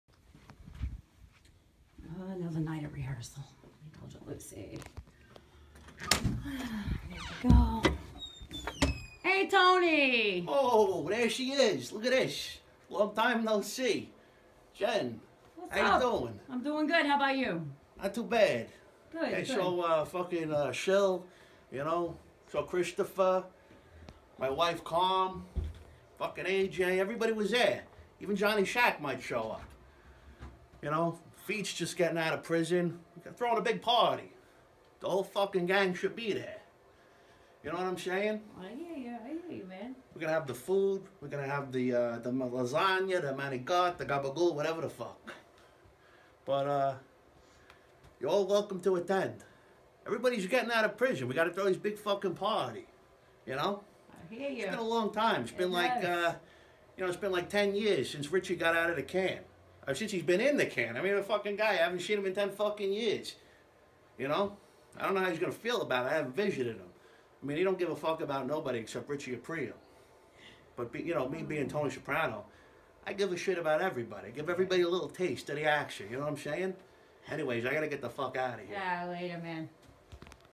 Tony saprano
Impression by indulgent lucies sax player Neil deRiggi